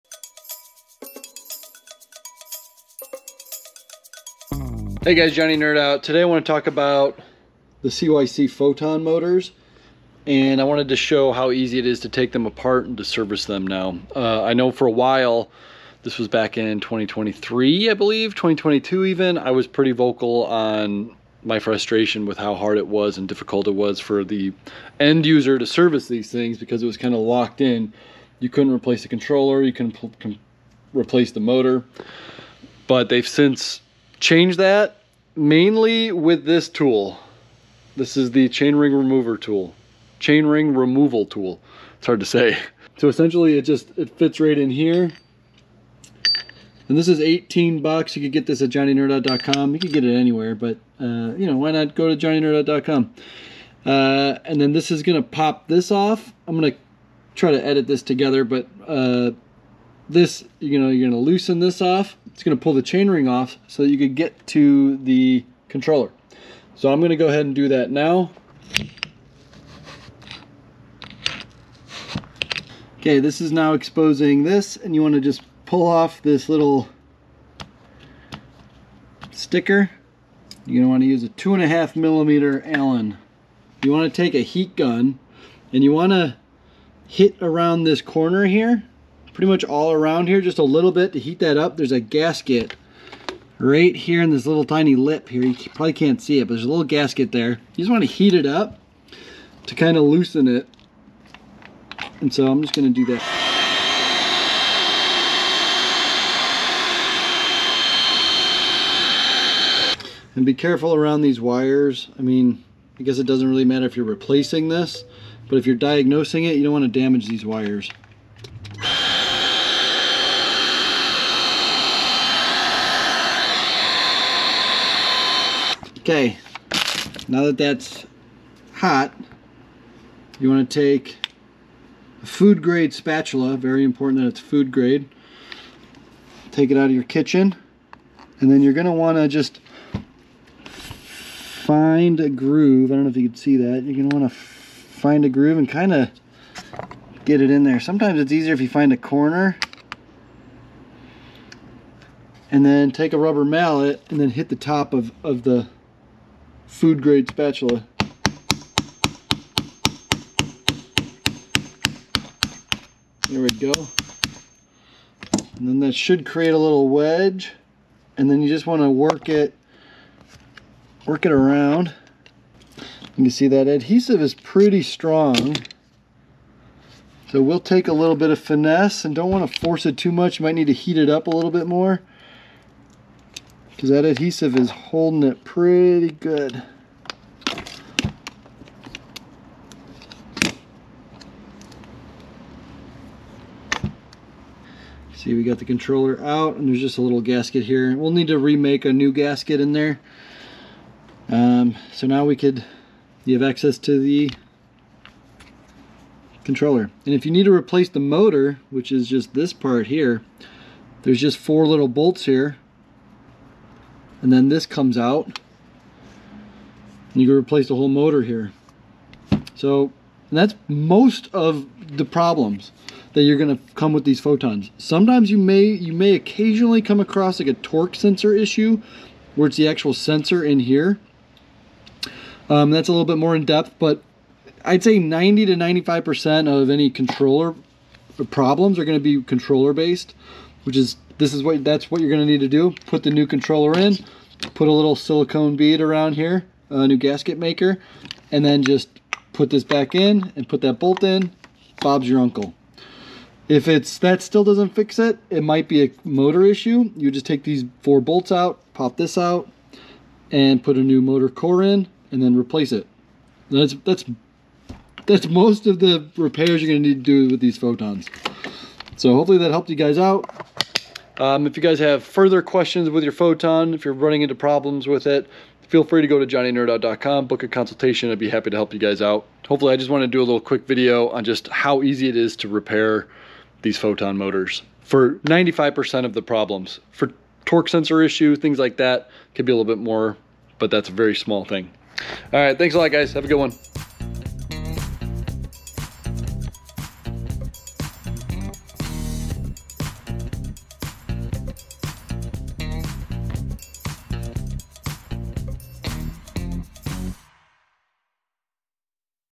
How to Easily open and repair your CYC Photon motor
for all your custom E-bike parts and help!